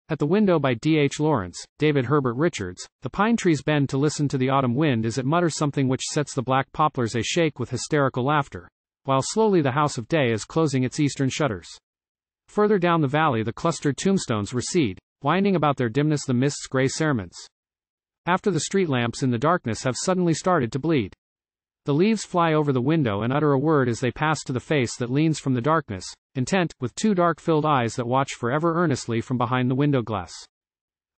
At The Window by D. H. Lawrence David Herbert Richards
The pine-trees bend to listen to the autumn wind as it mutters

    Something which sets the black poplars ashake with hysterical laughter;

    While slowly the house of day is closing its eastern shutters.

    Further down the valley the clustered tombstones recede,

    Winding about their dimness the mist's grey cerements, after

    The street lamps in the darkness have suddenly started to bleed.

    The leaves fly over the window and utter a word as they pass

    To the face that leans from the darkness, intent, with two dark-filled eyes

    That watch for ever earnestly from behind the window glass.